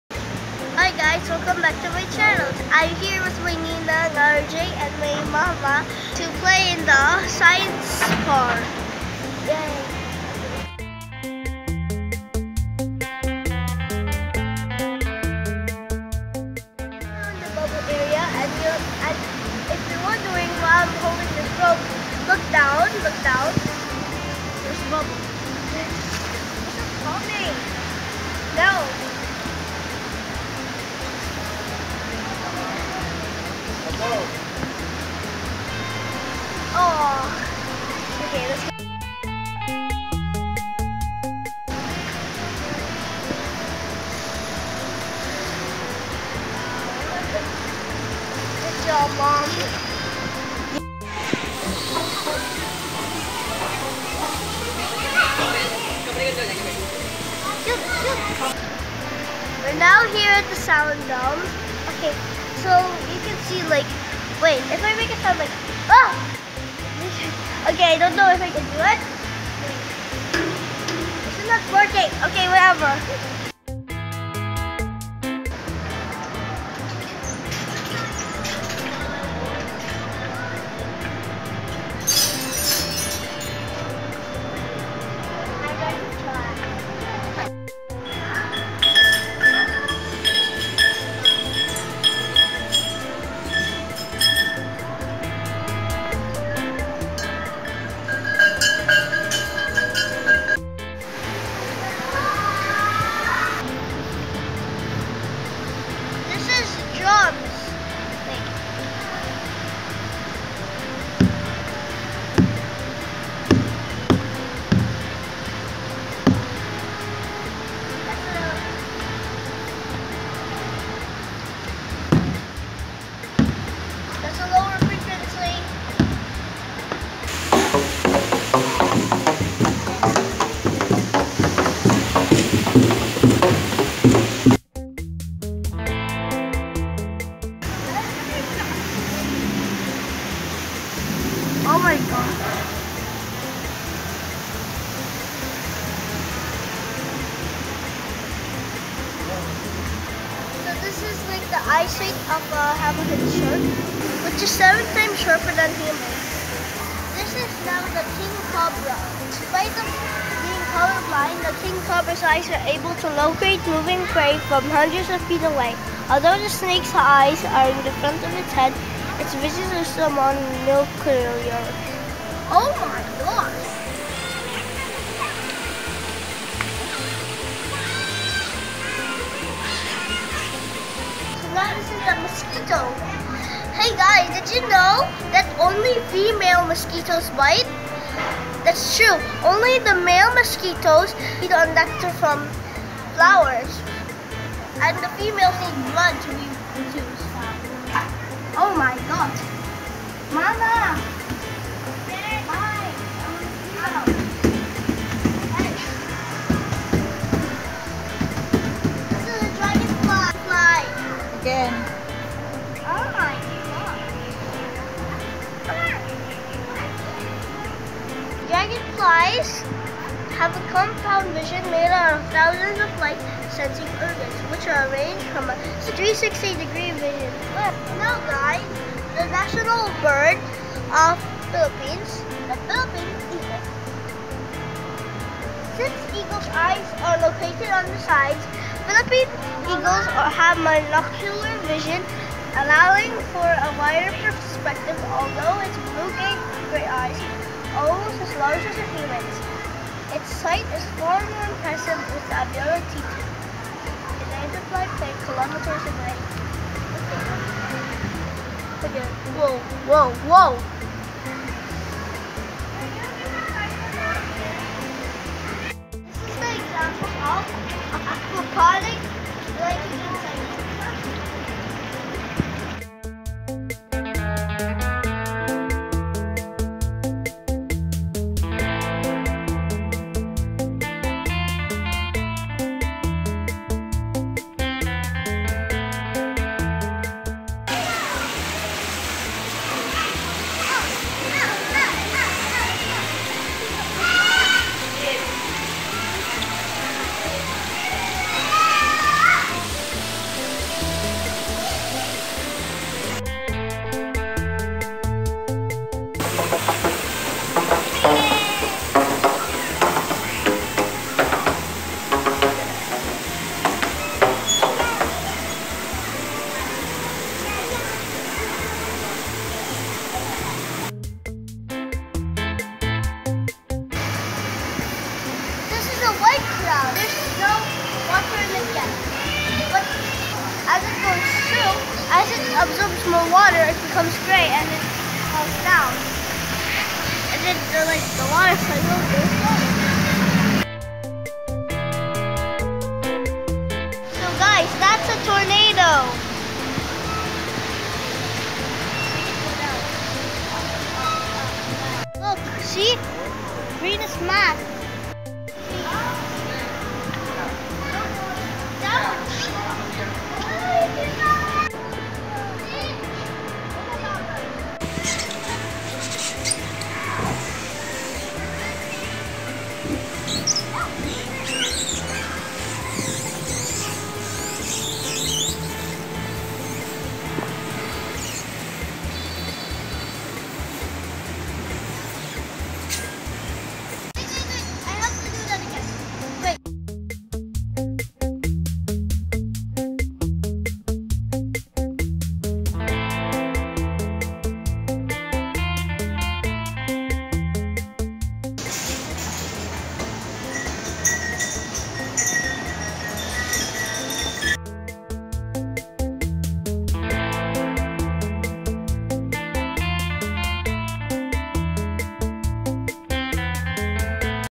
Science in the Park Tour | Uriel TV
While still in Manila, we got to visit one of the popular museums in BGC. I had fun exploring the park first while waiting for our chosen time. This is free when you purchase ticket of the museum but if not you have to pay 100php.